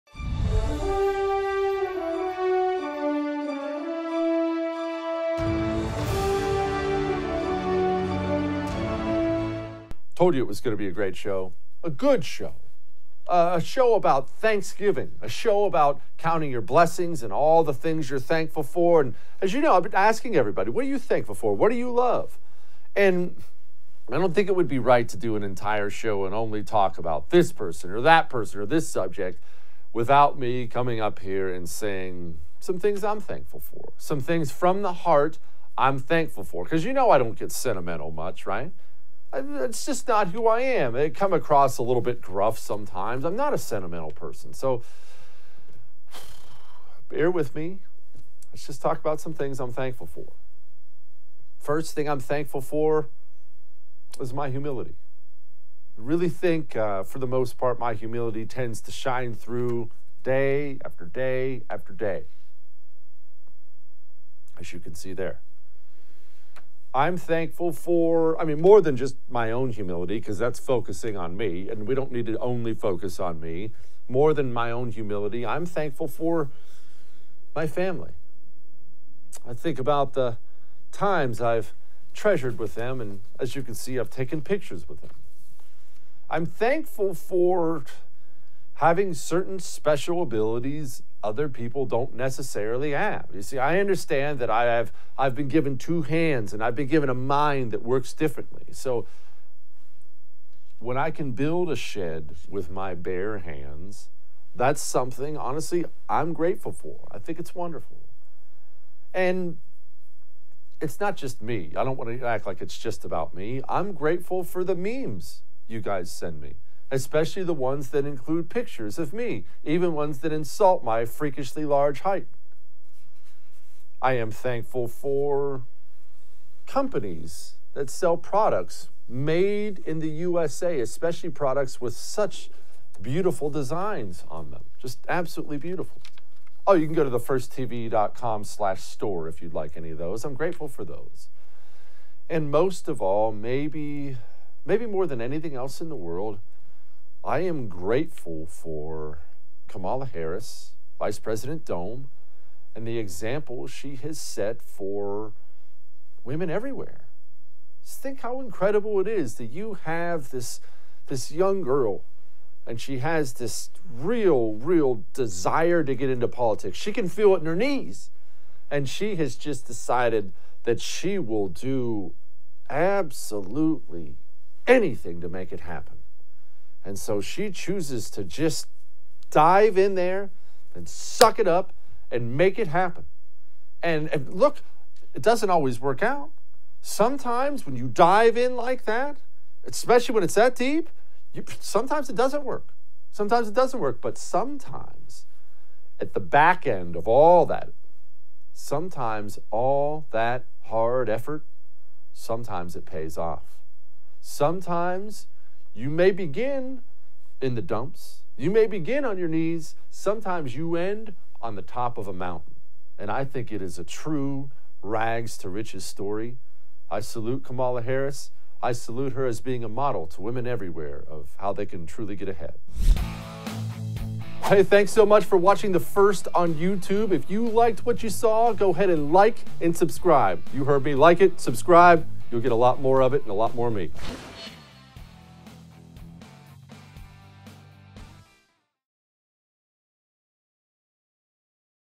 What Jesse Kelly Is Most Thankful For
Jesse Kelly pays tribute to one of the most humble, talented, and family-oriented people he's ever met. Yes, he's talking about himself. But that's not all. Jesse is thankful for one leading woman in America who gave up everything to achieve her goals.